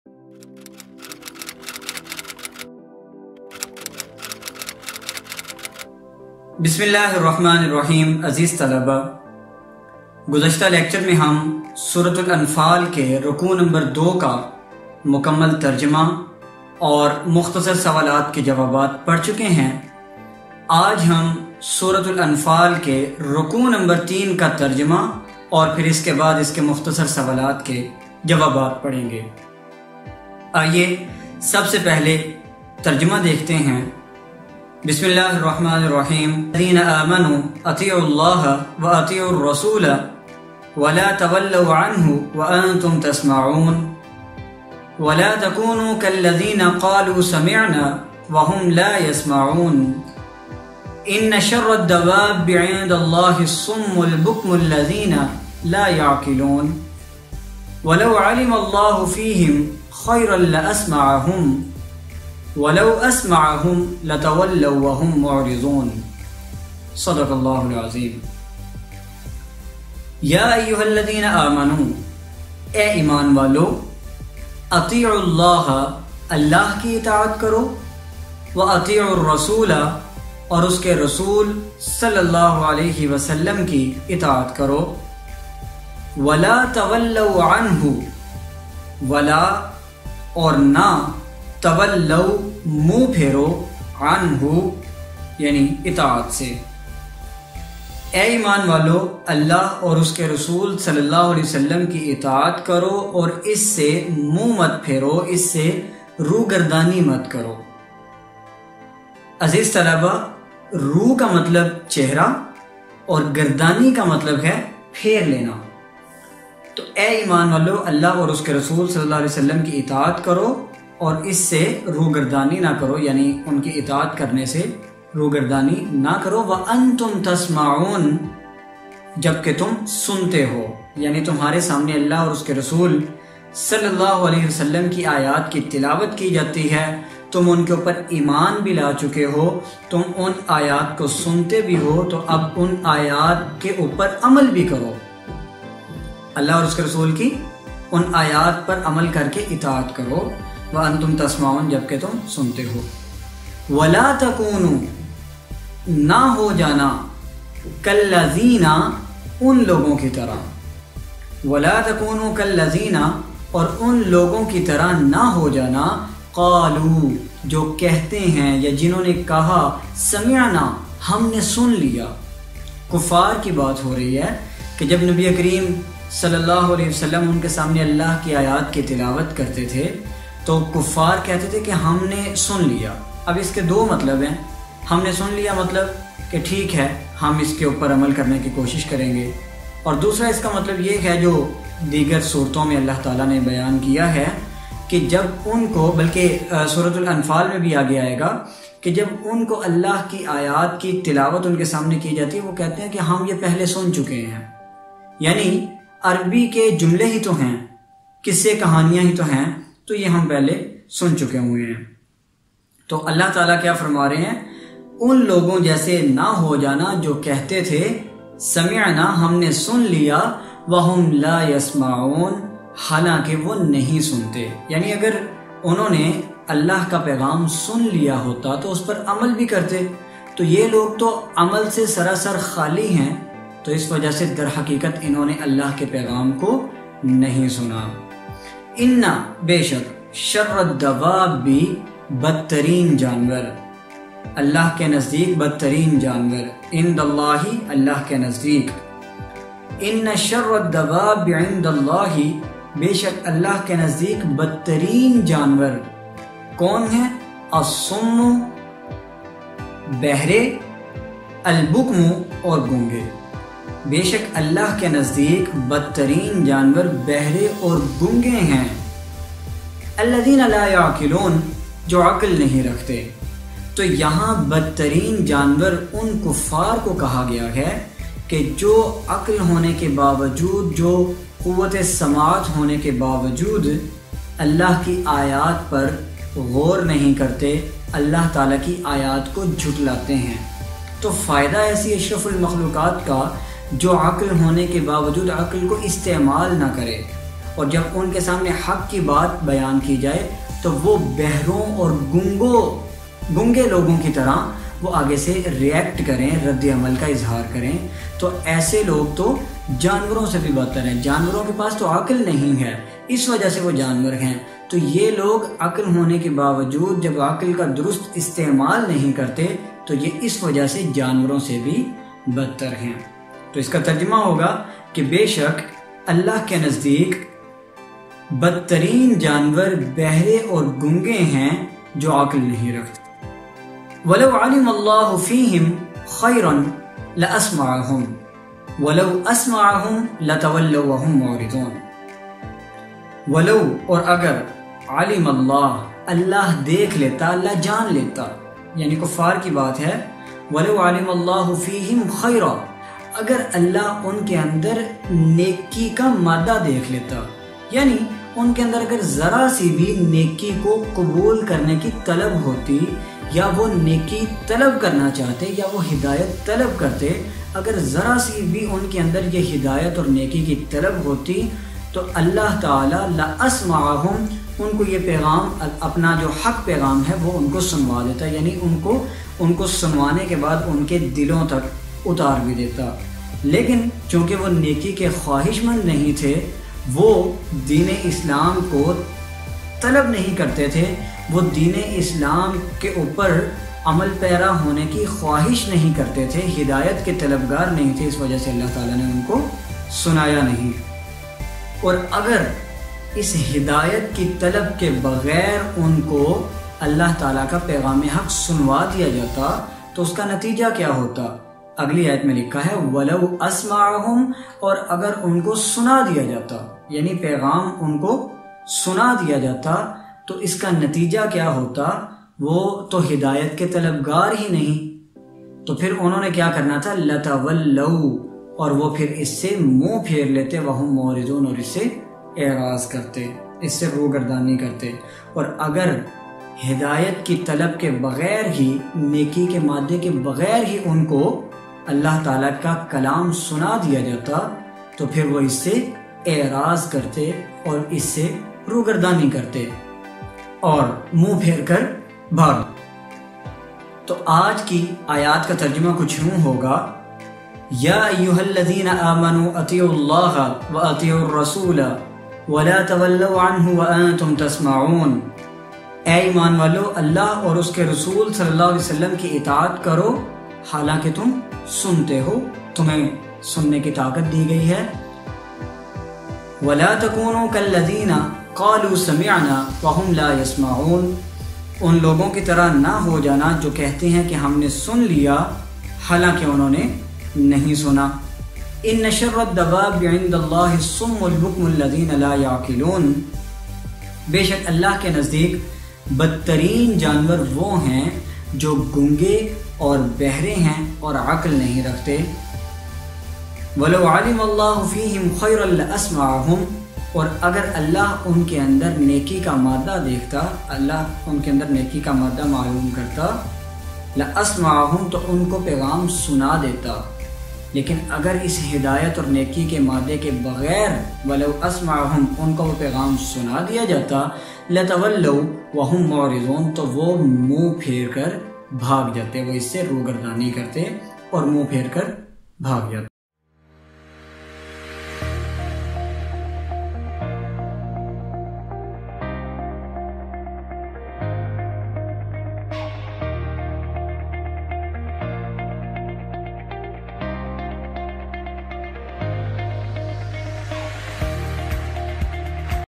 9th Islamiyat, Surah Anfal Rukoo No. 3 Part 1/2 Ayat 20 to 23 Translation |MANNAN EDUCATION
Islamiat Links
All about Matric and Intermediat Urdu, Pak Studies and Islamic Studies Solution, in this lecture i will teach you Translation and Explanation of Surah Al_Anfal Rukoo No. 2 Ayaat 15 to 19 (Islamiyat Class 9th) Subscribe,Like, Share and Comment
By
 Hafiz Abdul Mannan Aziz
(M.Phil)
University of the Punjab